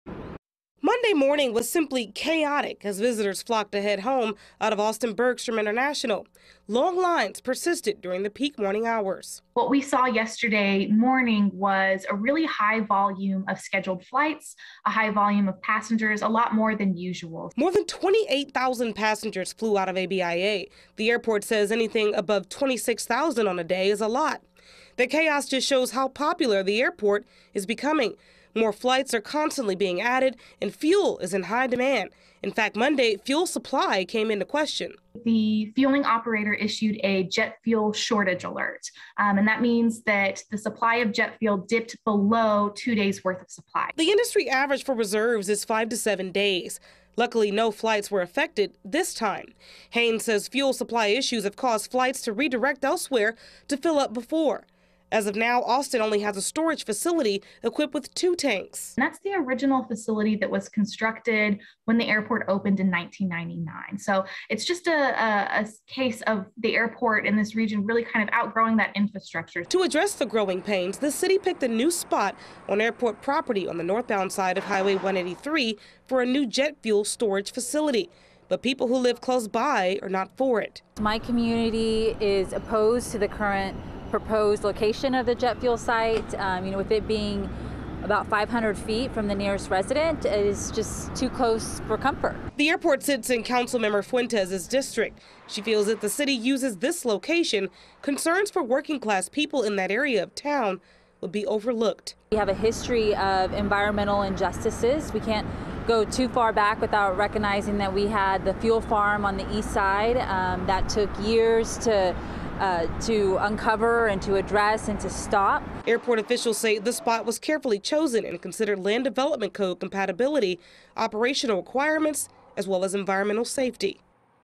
Austin-Bergstrom International seeing major growing pains | FOX 7 Austin
Austin-Bergstrom International Airport is seeing more flights, more destinations and more people, but now Austin City Council has to approve more fuel.